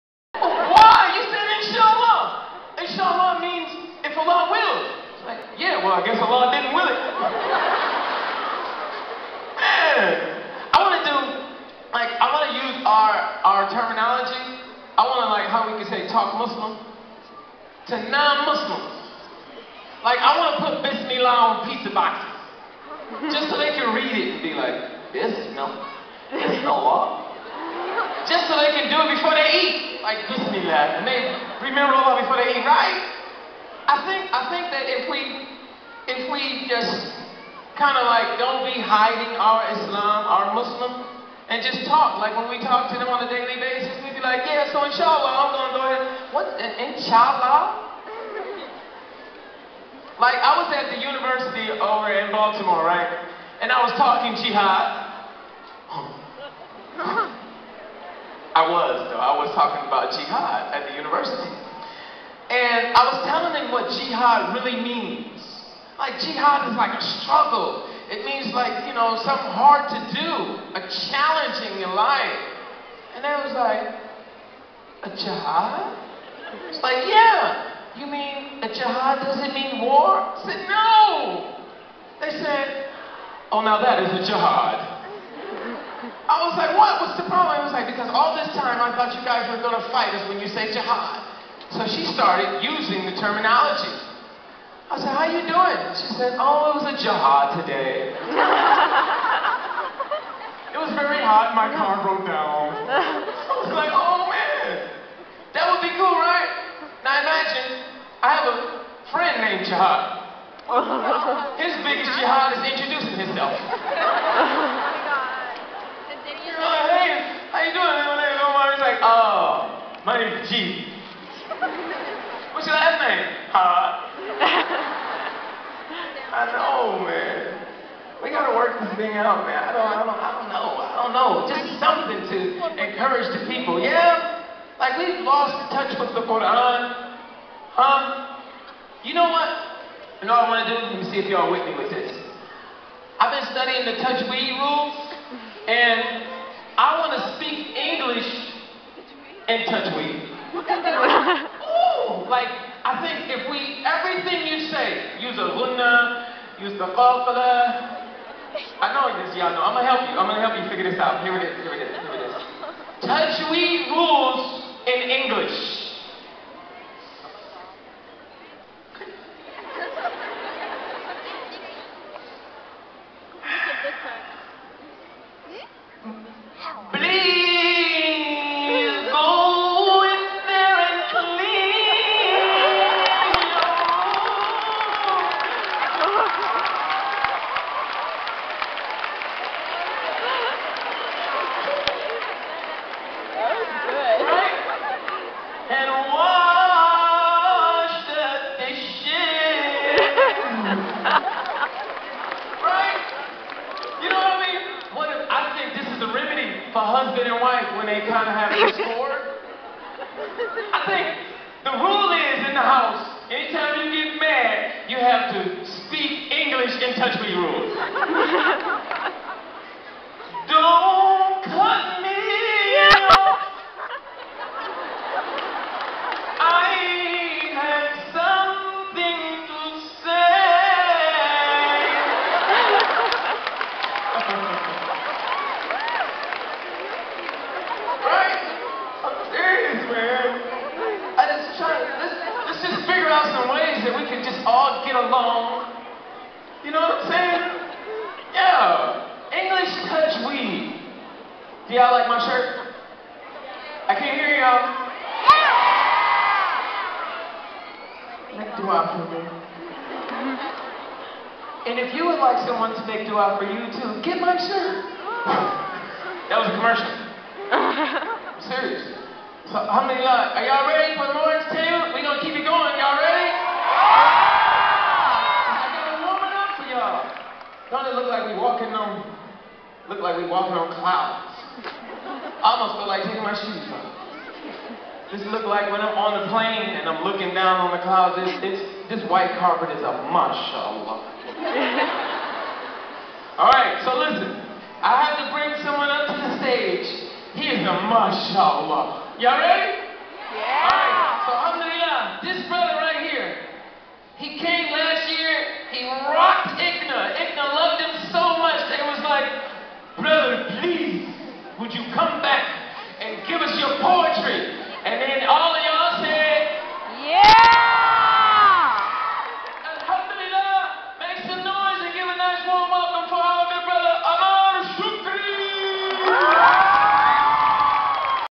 Omar Regan ICNA 2012
Omar Regan on May 27th emceeing the entertainment session at the Islamic Circle of North America 2012 convention held in Hartford, CT!